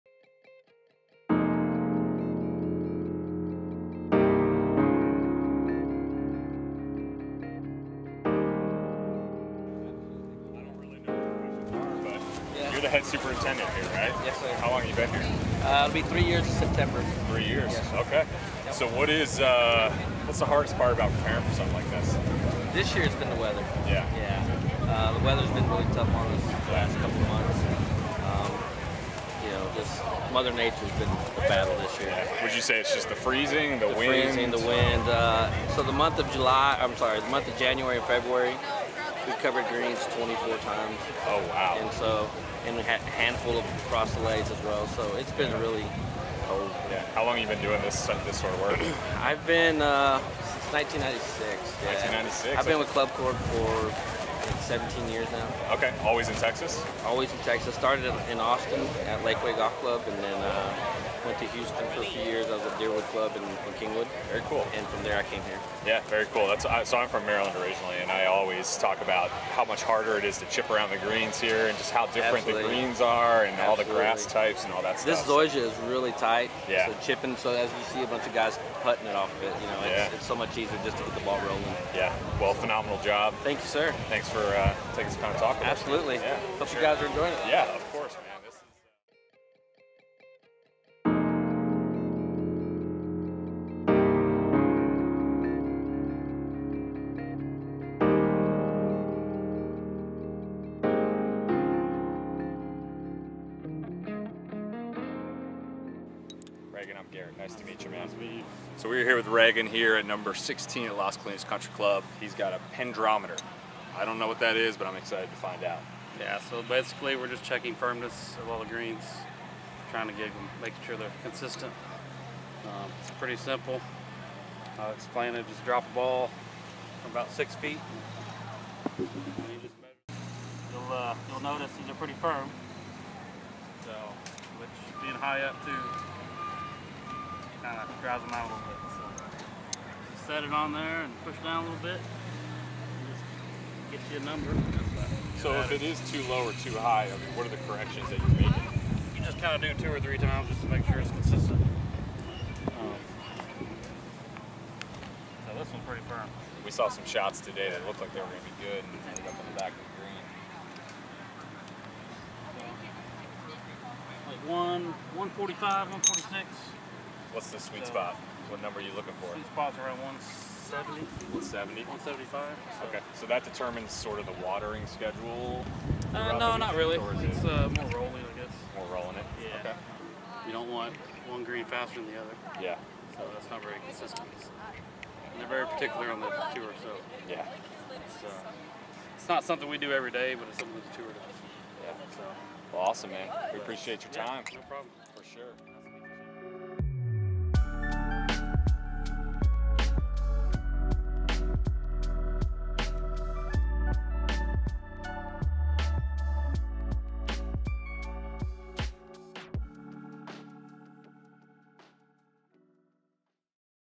The Unsung Heroes of the Golf Course
We're at the ClubCorp Classic this week on the grounds with some of the awesome maintenance staff that help make this course tournament ready.